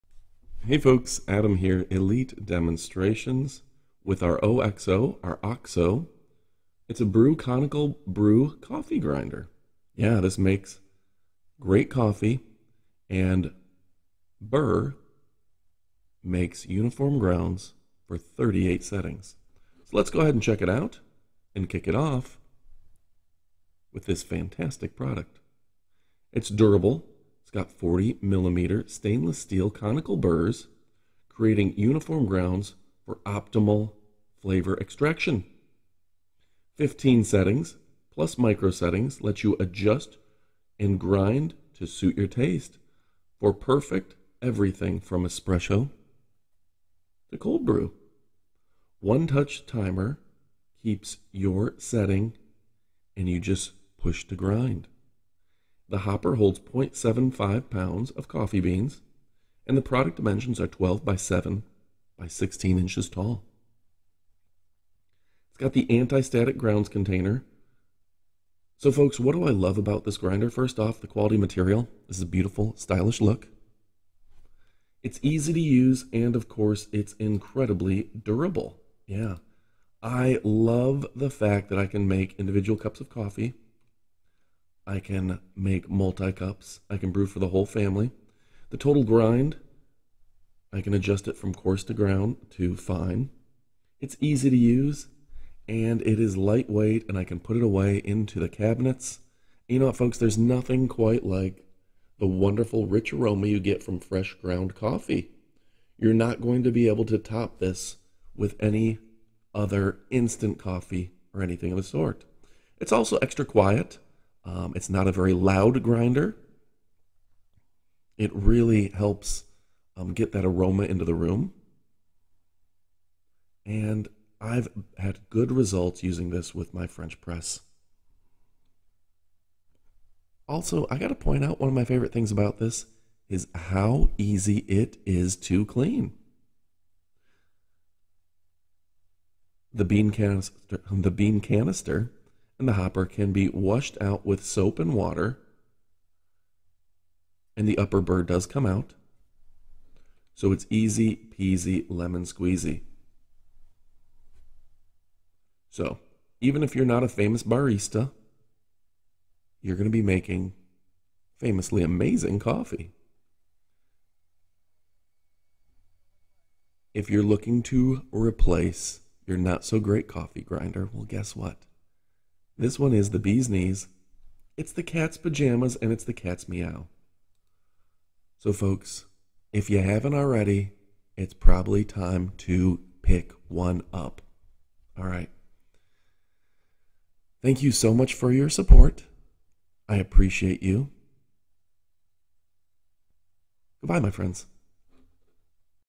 OXO Brew Conical Burr Coffee Grinder
Durable 40 millimeter stainless steel conical burrs create uniform grounds for optimal flavor extraction
    15 settings (plus Micro settings) let you adjust your grind to suit your taste. Perfect for everything from espresso to cold brew
    One touch start timer keeps your last setting, just push to grind
    Hopper holds up to 0.75 pounds of Coffee beans. Product Dimension - 12x7.4x16 inch
    Anti-Static grounds container accommodates up to 110 grams (enough for 12 cups). Never use water or other liquids to clean the inside of the grinder/burrs